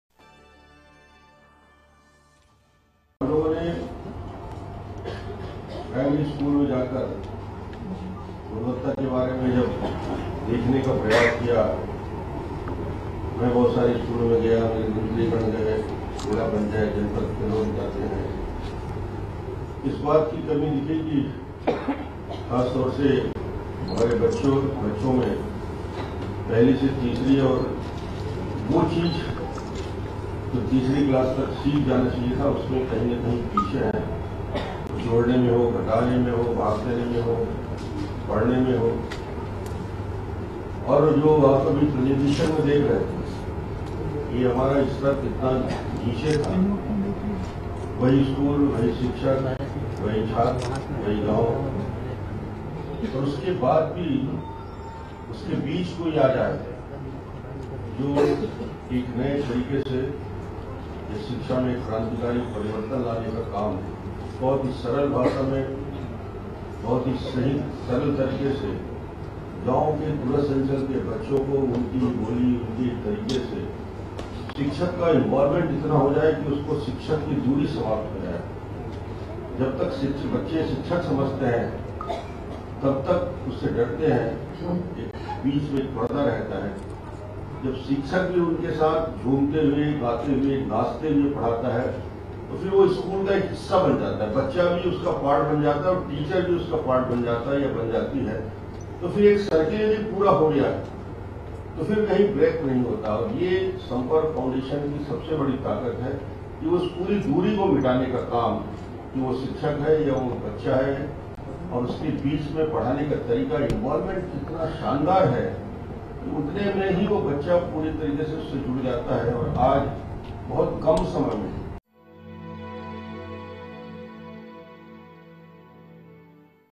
CM Raman Singh: Devise New Methods of Teaching to Improve Quality of Education
Speaking during the launch of the "Abdul Kalam Quality Education Campaign" Chhattisgarh Chief Minister Raman Singh on Tuesday said that more than one lakh teachers have already been given skill development training to improve their teaching skills under the campaign, but still teaching standards needed to improve. He said he and his cabinet colleagues have decided to review the standard of education in primary schools in the state.